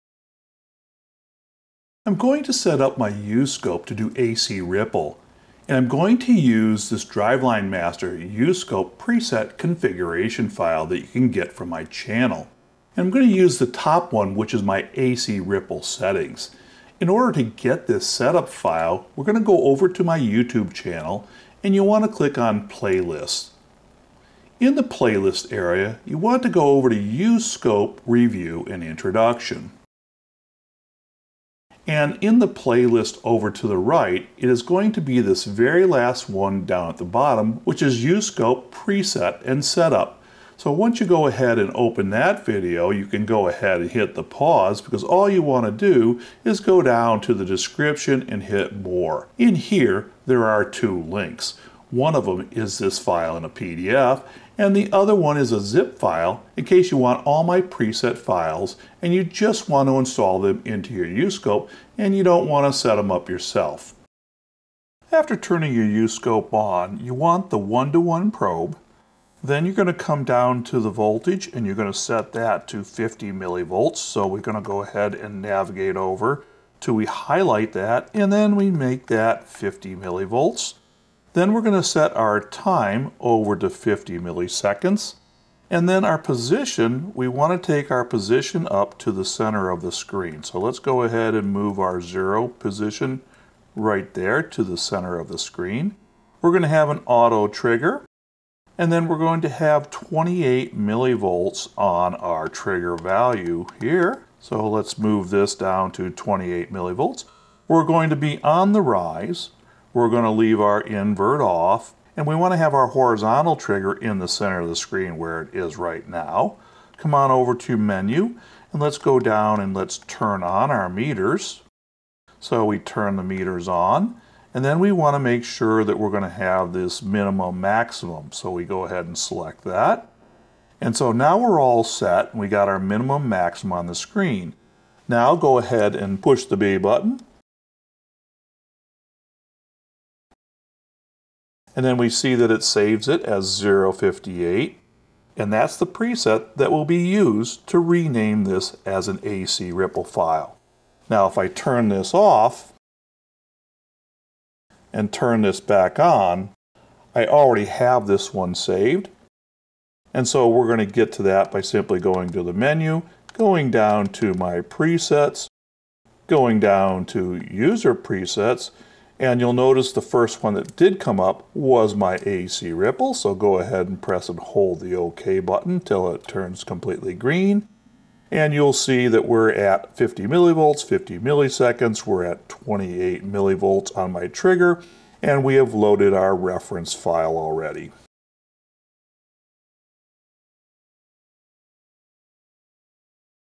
Oscilloscope AC Ripple Configuration Settings (uScope)
How to setup your uScope Oscilloscope to measure AC Ripple on a vehicle.